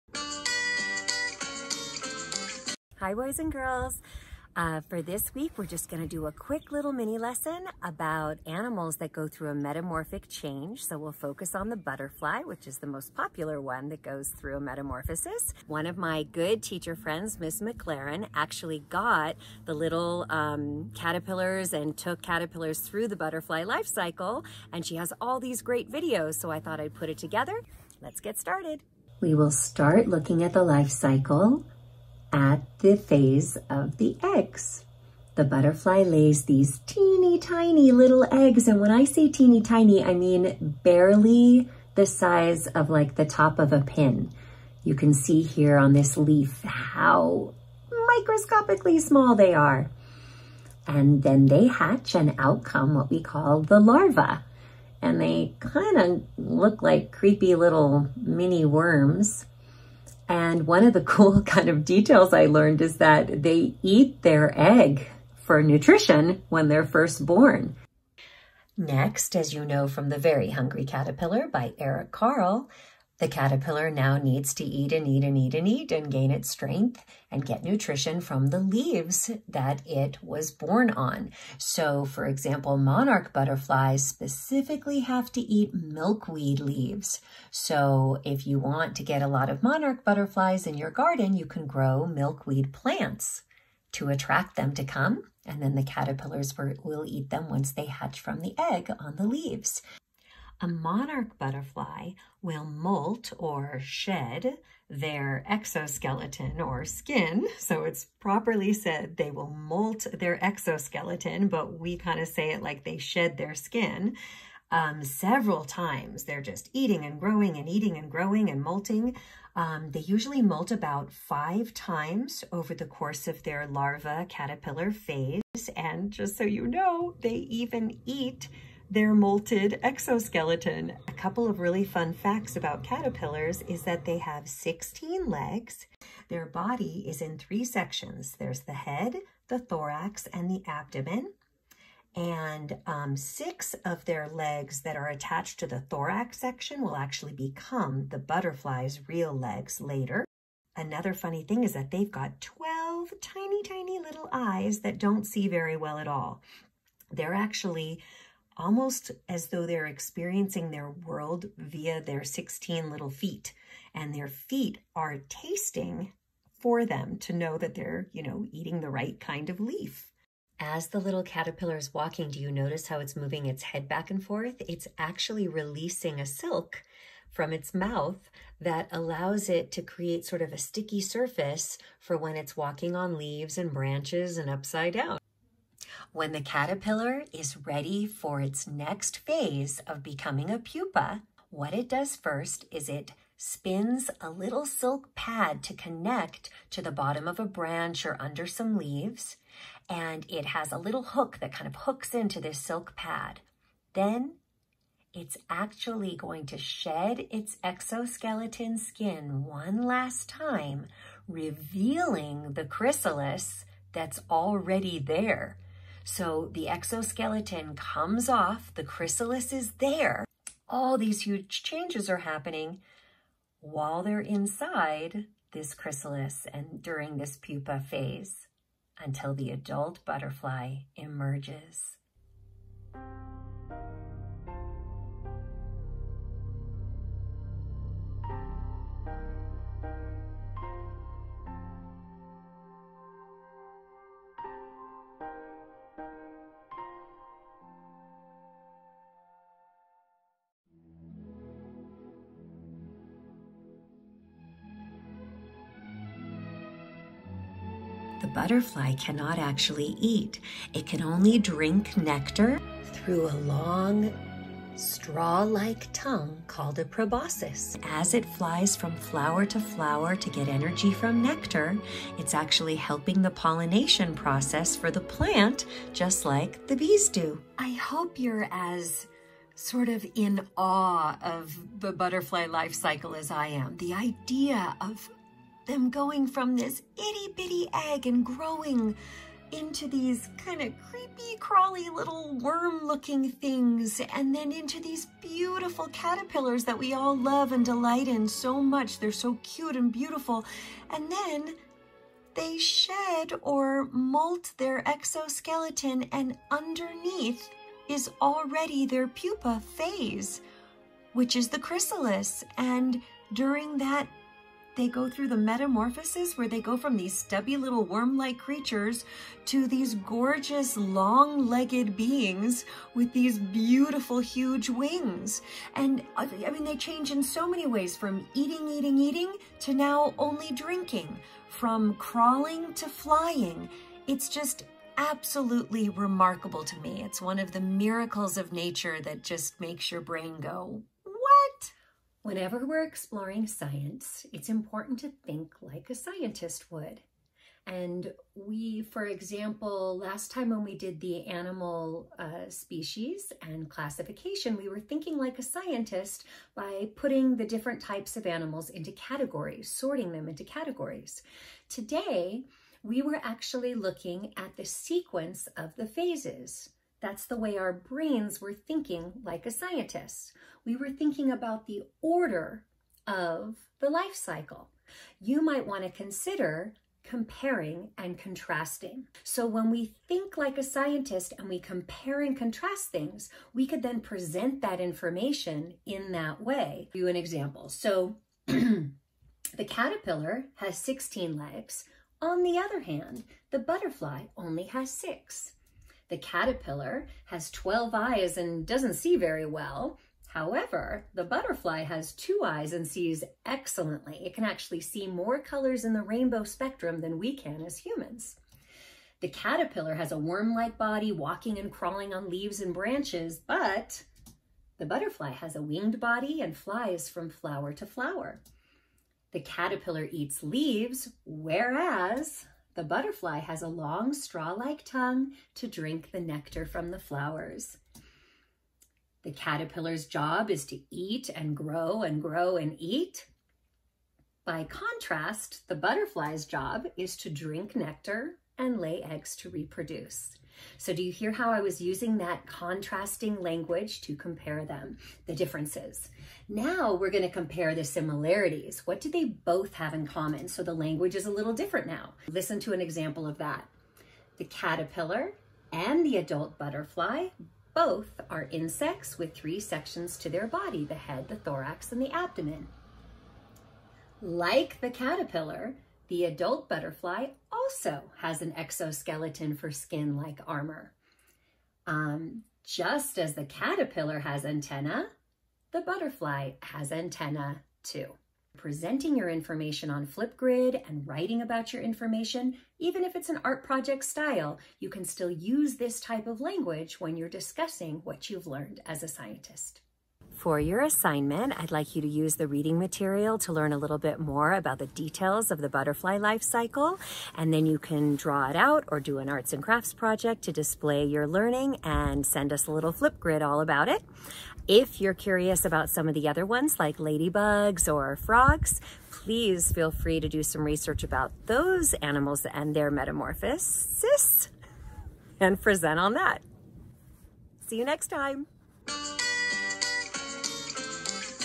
Science in Nature, Butterfly Metamorphosis
This Science in Nature video is about the butterfly life cycle and an introduction to metamorphosis. It also includes an ELD mini-lesson on the language of comparing and contrasting.